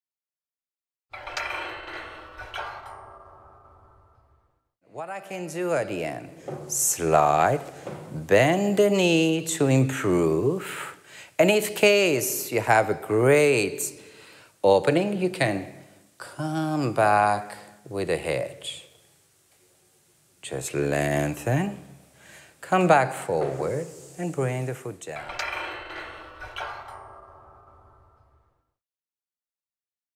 2 Keys to Easy Backbends PREVIEW
/ In this teaching tips video, Fabien demonstrates a progression of exercises that address the 2 key areas of the body needed for healthy backbends. They are 1) the flexibility and strength of the lower trunk, and 2) the flexibility and strength of the upper trunk. Without strength and flexibility in these 2 areas, you are likely to take all of the bending in your lower back.

For private lessons visit Fabien’s studio Centre Pilates Barcelona and for teacher training with Fabien and Miguel Silva in Spain and Portugal, visit Uno Pilates.